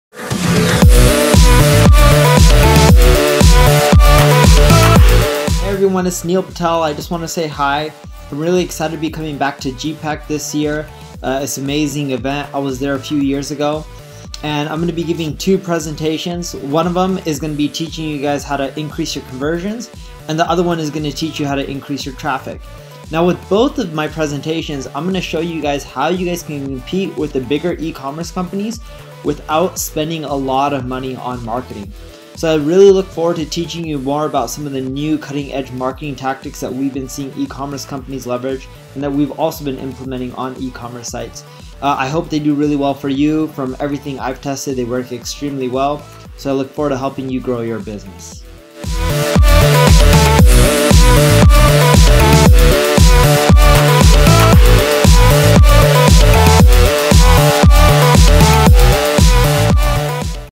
Neil Patel va vorbi la GPeC SUMMIT despre cum să crești traficul și rata de conversie
Neil Patel este Co-Fondatorul Crazy Egg, Hello Bar, KISSmetrics și un adevărat geniu în Marketing Online. Neil va susține un keynote de 45 de minute în cadrul GPeC SUMMIT pe 14 noiembrie, când va dezvălui audienței 7 secrete pentru dublarea ratei de conversie în e-commerce, iar pe 15 noiembrie va avea o sesiune practică de 90 de minute în cadrul GPeC Special Day.

Mai mult de 30 de speakeri internaționali și români sunt anunțați în 2017 la GPeC SUMMIT – Cel Mai Important Eveniment de E-Commerce și Marketing Online, care se va desfășura la București, în zilele de 14 și 15 noiembrie.

Înscrierile se încheie pe 10 noiembrie, dar, până în 31 octombrie - ora 23:59, organizatorii au pregătit o ofertă specială de preț în campania Early-Bird, de doar 109 euro + TVA, pentru ambele zile de eveniment. Începând cu 1 noiembrie, prețul devine 149 de euro. Detalii și înscrieri